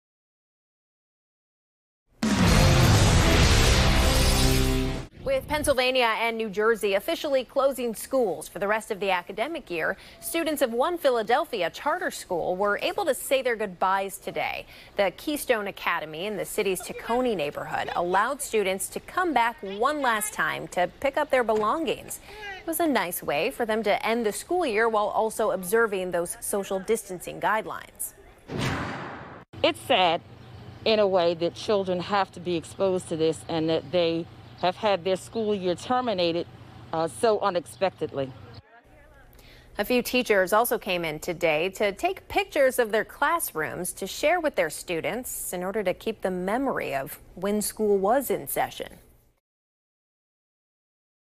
Keystone Academy Students Return for Belongings
Keystone Academy Charter School students returned on May 4 to pick up their belongings left behind in March when schools across the state closed without warning.